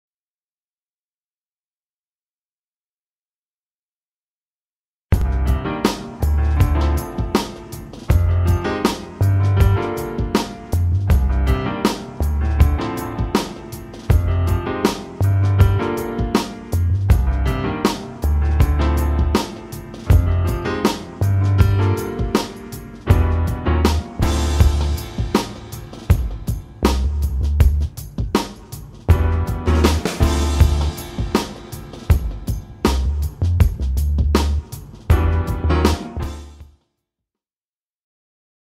Volkswagen golf iv occasion visible à Toulouse présentée par Automobiles regis guillard
Cette Volkswagen golf iv occasion disponible à Toulouse dans le 31 est mise en vente par Automobiles regis guillard - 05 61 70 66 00 - tous les détails sont